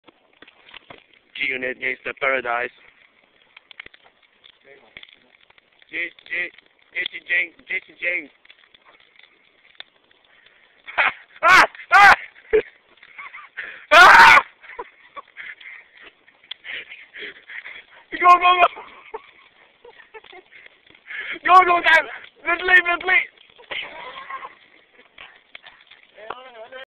shovel shot on jessy james face! wake up
Jessy james is drunk. He needs to wake up before he takes many drugs. WAKE UP, REVEILLE TOI SAKOUP!